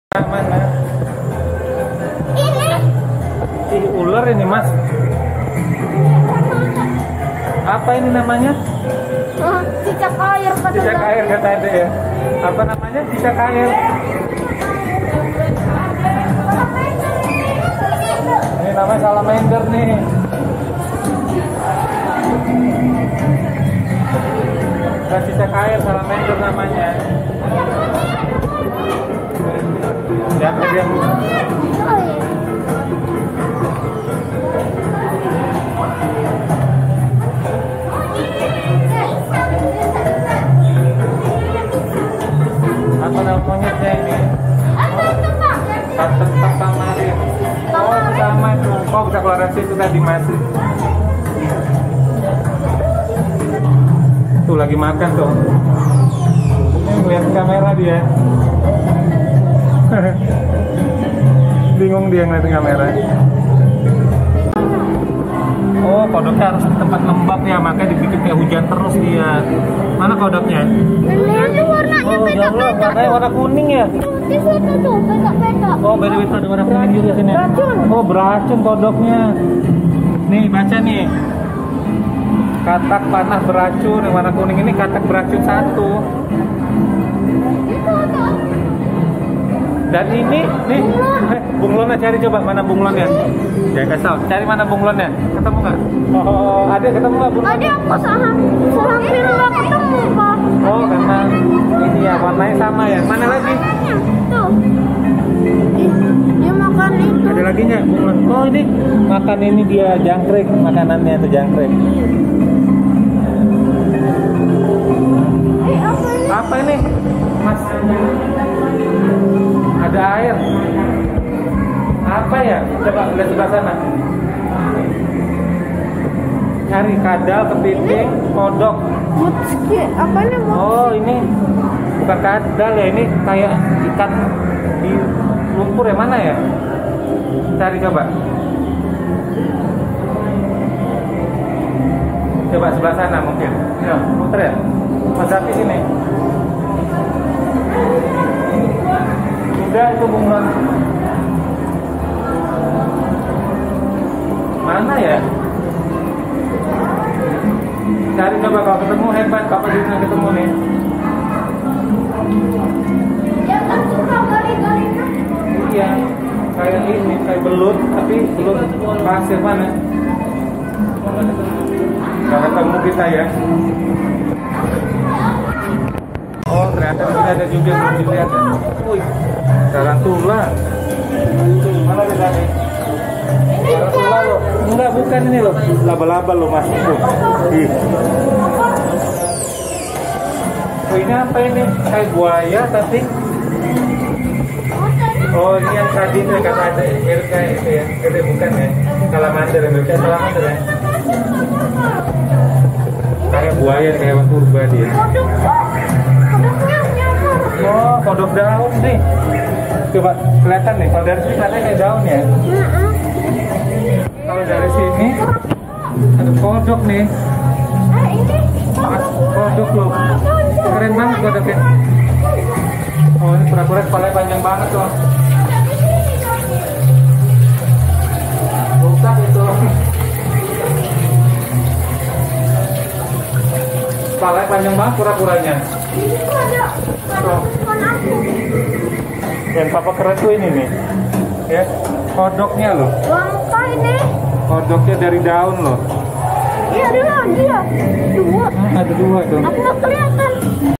Mas Abi melihat monyet aneh dan kodok yang mirip banget dengan daun
Jakarta aquarium punya banyak hewan menarik untuk dilihat